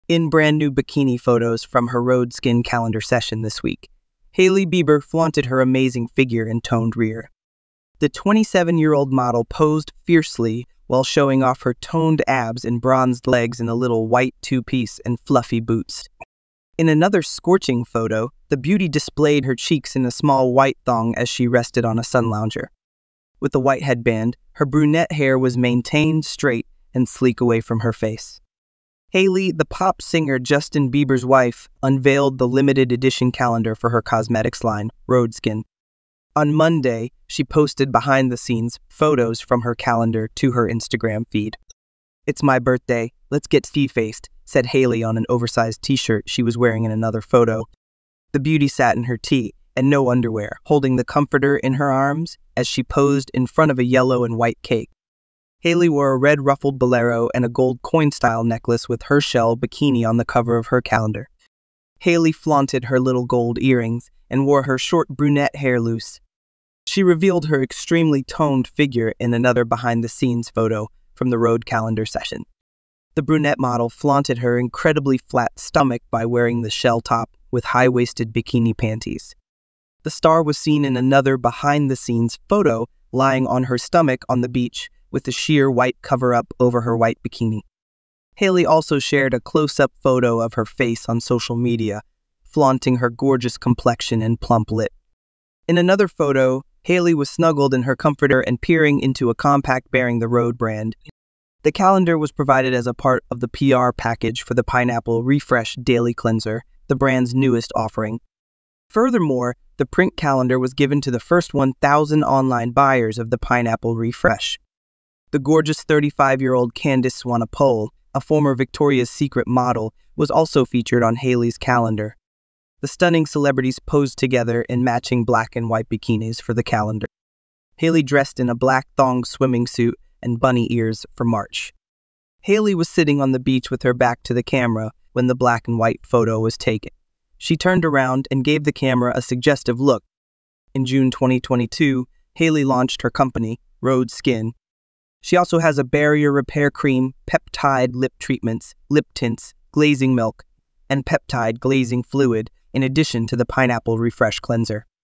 Hailey Bieber’s Rhode Skin Calendar: Behind the Scenes
Join Hailey Bieber as she takes you behind the scenes of her Rhode Skin calendar shoot, featuring stunning bikini snaps and exclusive beauty tips. The model and entrepreneur shows off her incredible physique and flawless skin in a variety of outfits, from a white thong bikini and fluffy boots to a shell bikini and a red bolero. She also reveals the secret to her radiant glow: the Pineapple Refresh daily cleanser, the newest product from her beauty brand Rhode Skin. Watch the video to see how Hailey and her friend Candice Swanepoel rocked the Rhode Skin calendar and get your own copy of the limited-edition calendar with your purchase of the Pineapple Refresh online. Don't miss this chance to see Hailey Bieber like never before!